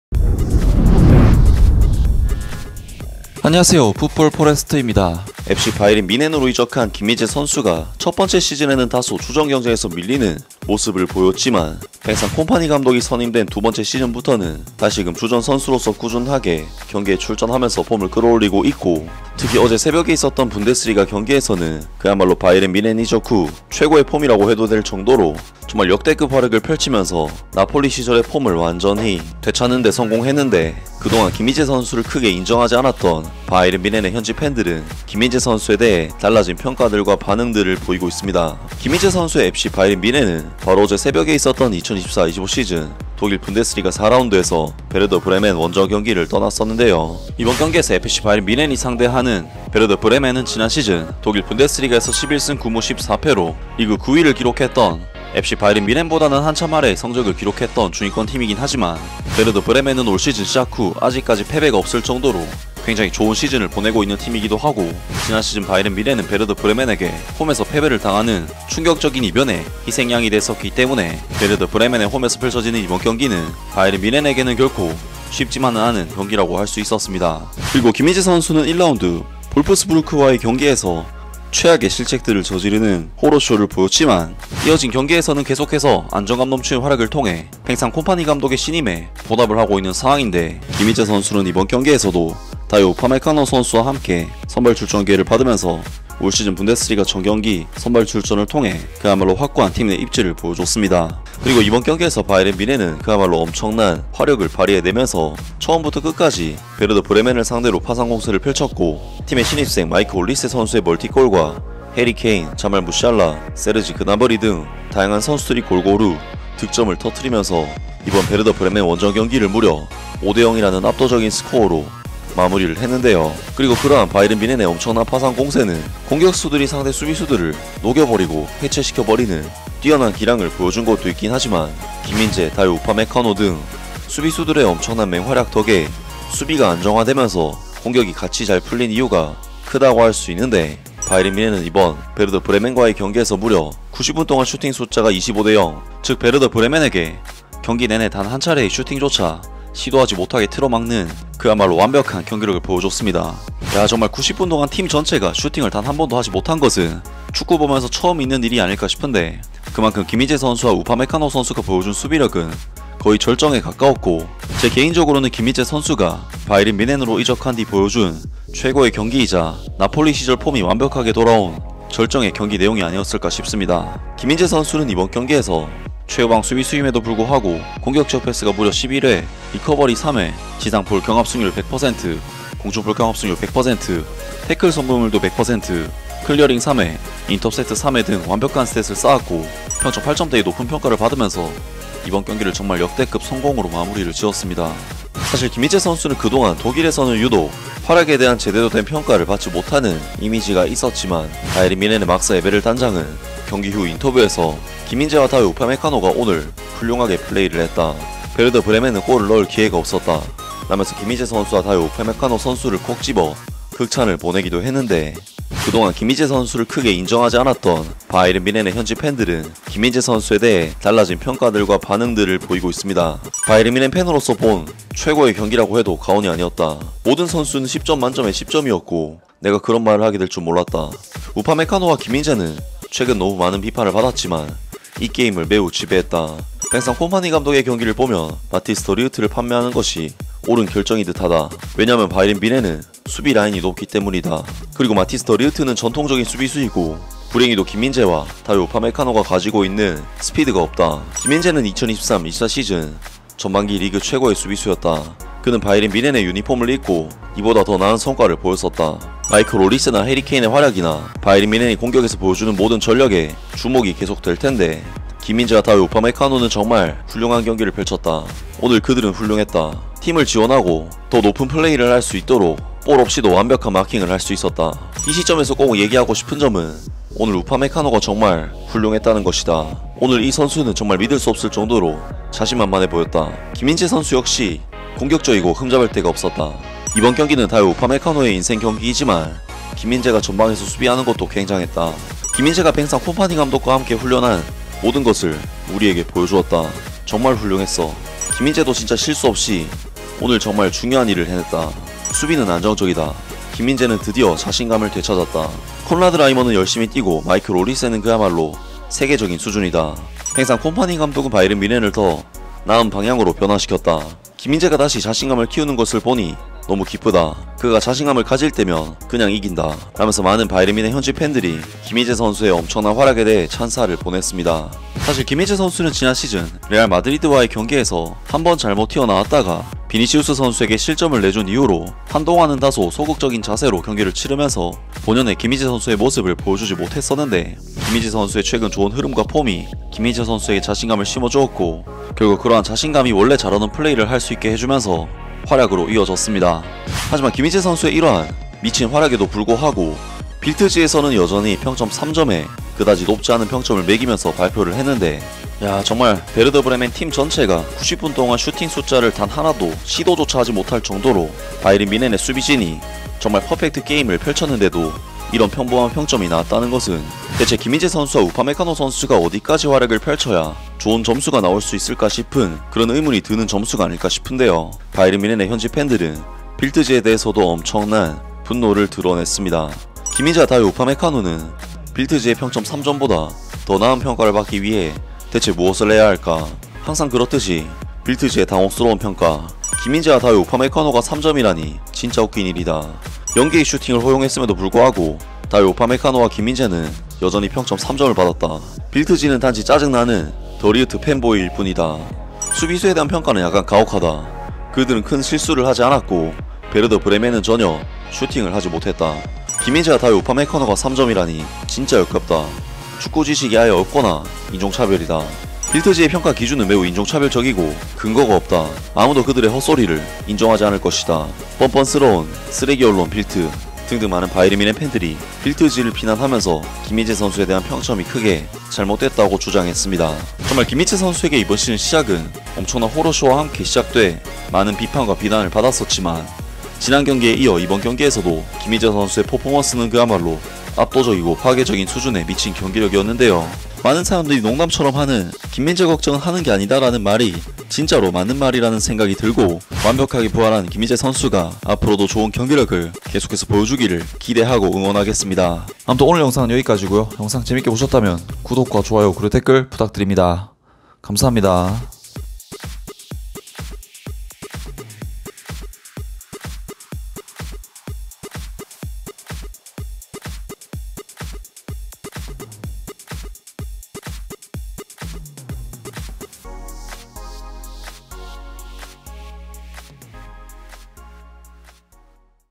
"김민재 달라진 것 같다" 김민재 뮌헨 현지 팬들 여론 드디어 역전됐다! 빌트 지 혹평에 분노 터진 뮌헨 팬들 반응 "빌트지는 쓰레기 언론" [뮌헨 vs 브레멘]
구독!